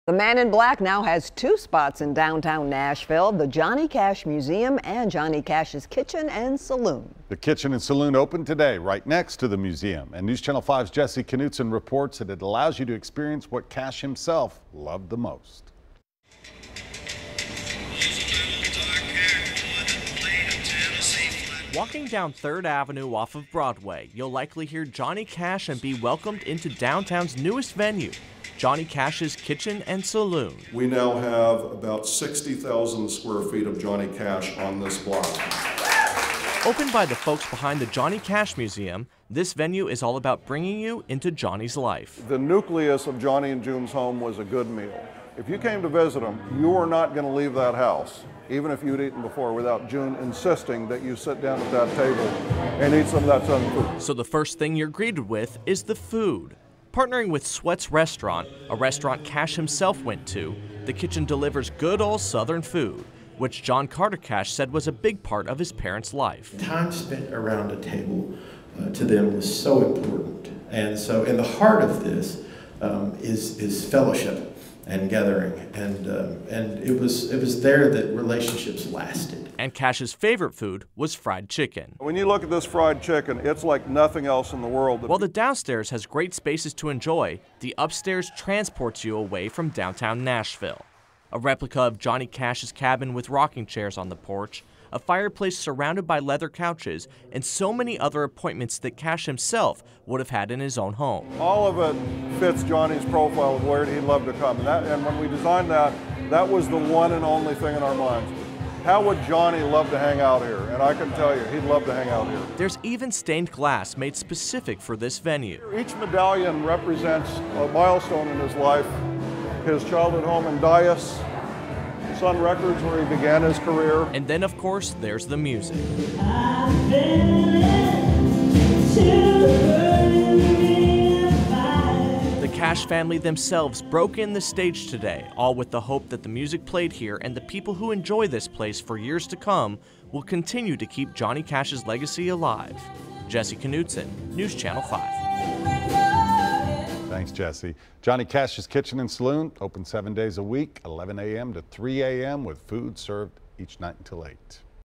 Johnny Cash’s Kitchen and Saloon opens in Nashville
Johnny Cash’s Kitchen and Saloon opened right next to the Johnny Cash Museum on 3rd Avenue.